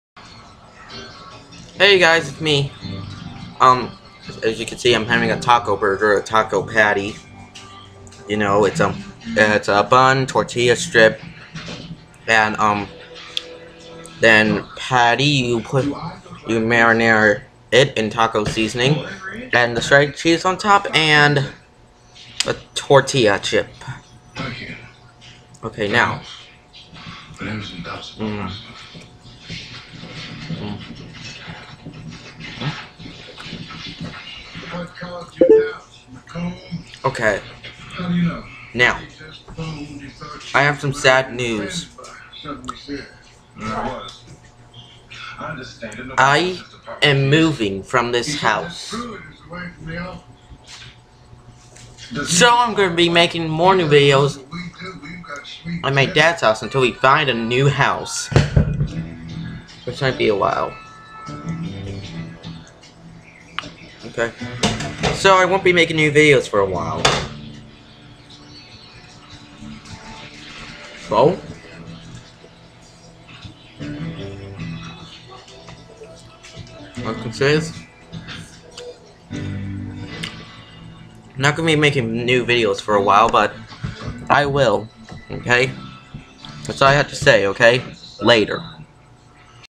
I'm gonna move
I'm moving out of my house so no new videos for a while.